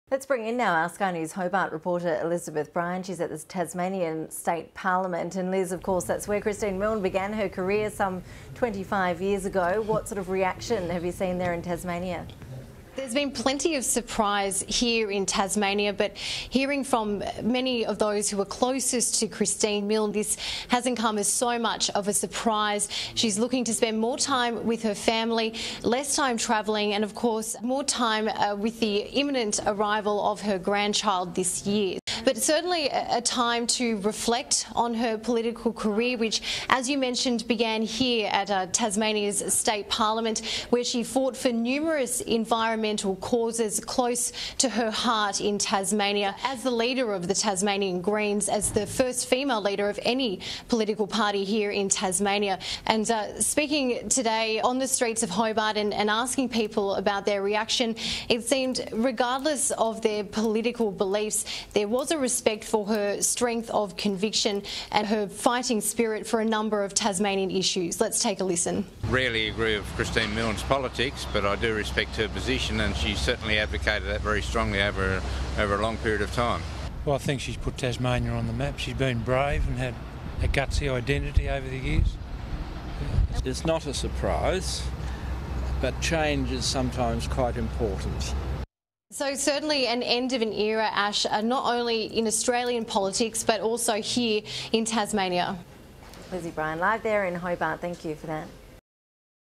Milne's suprise resignation
LIVE cross from Tasmanian parliament after Christine Milne resigns as leader of the Greens